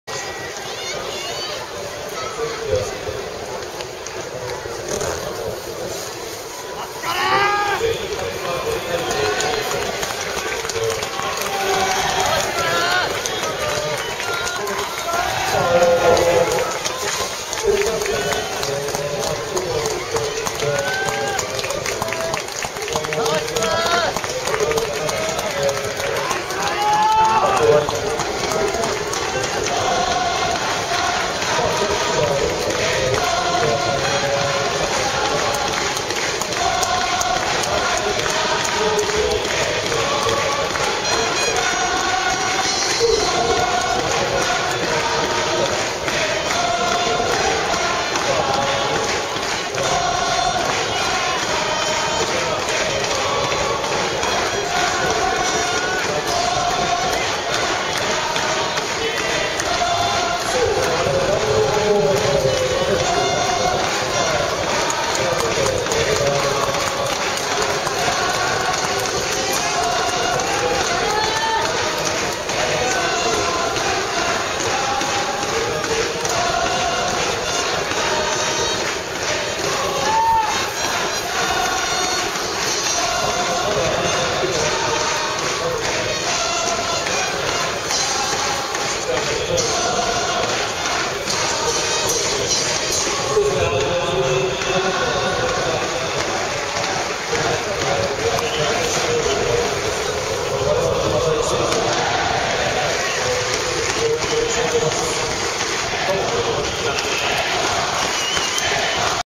雨の日のふろん太とカブ
雨の日仕様です。

2009/4/21
川崎フロンターレ－セントラルコーストマリナーズ（オーストラリア）

Todoroki sutadium,Japan
Kawasaki Frontale -  Central Coast Mariners